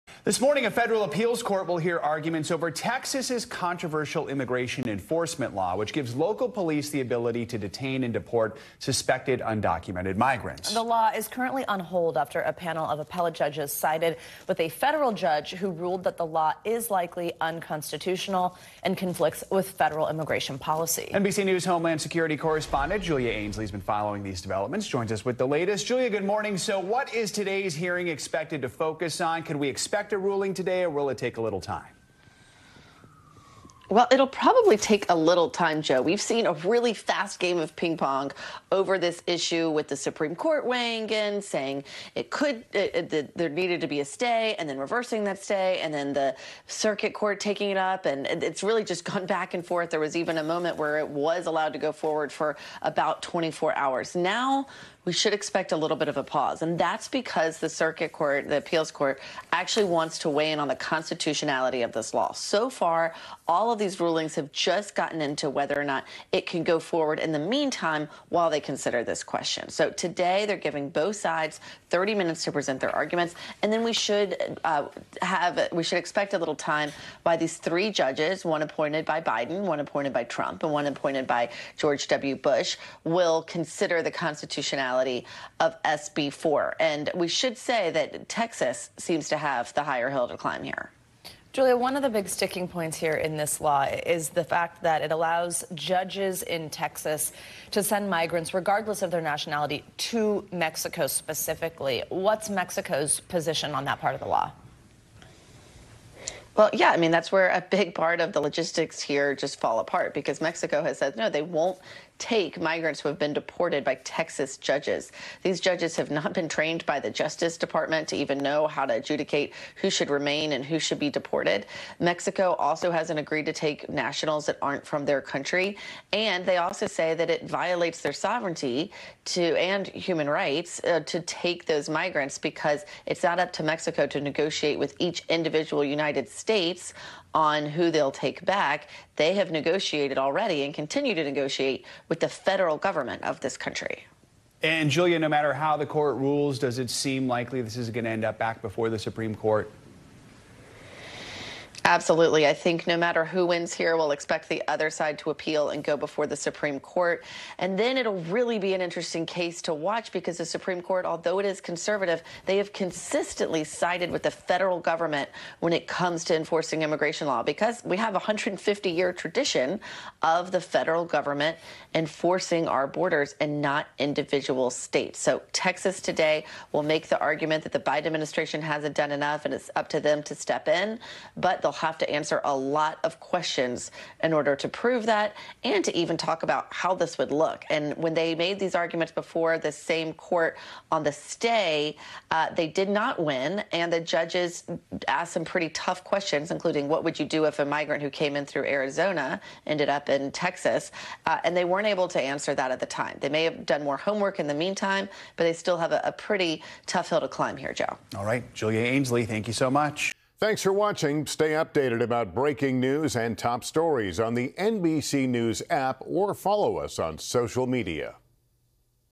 Appeals court to hear arguments over hold on Texas immigration law
The Fifth U.S. Circuit Court of Appeals will hear arguments on an appeal from Texas after the court blocked a state law that would give police the power to arrest migrants who enter the country illegally. NBC News’ Julia Ainsely reports on how the court will weigh the law’s constitutionally and if it could be argued before the Supreme Court.